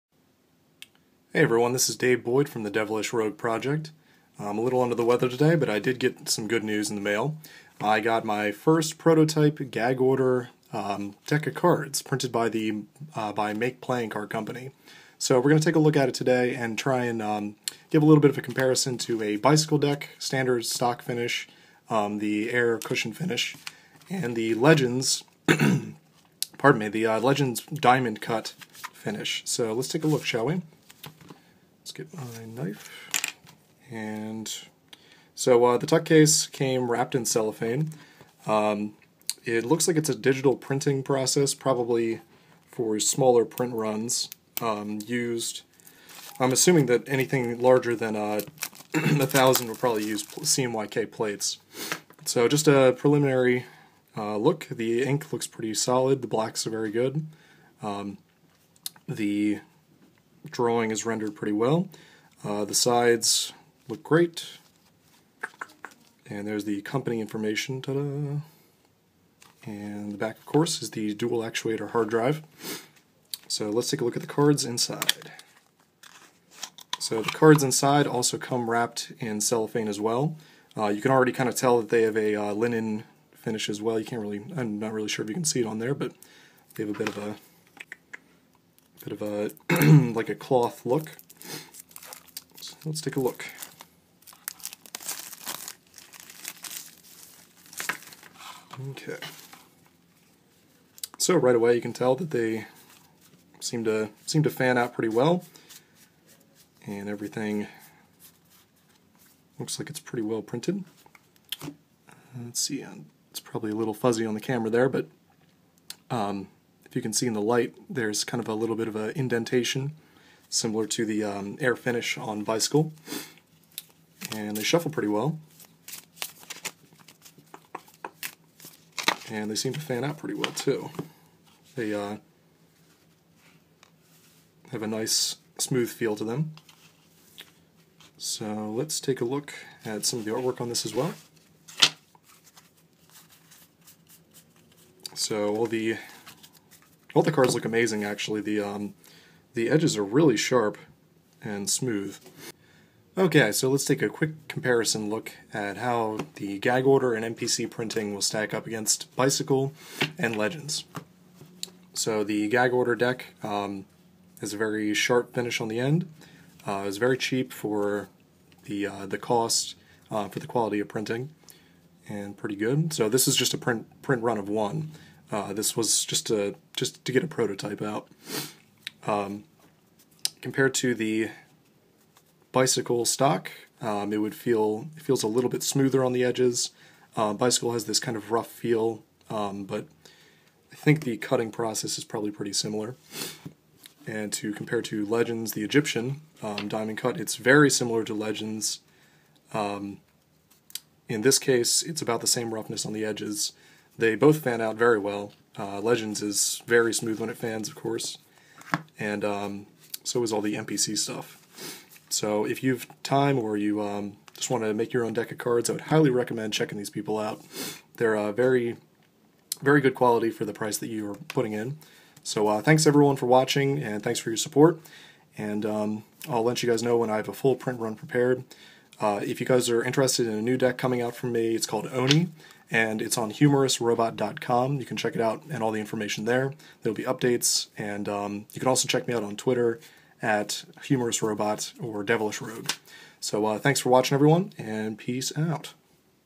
Gag Order Playing Cards: Unboxing
Video review of the Gag Order Playing Cards, printed by the Q. P. Printing Company,  aka MakePlayingCards (MPC). Gag Order is a spy-themed deck of cards based on an artist's rendering of a dystopian future.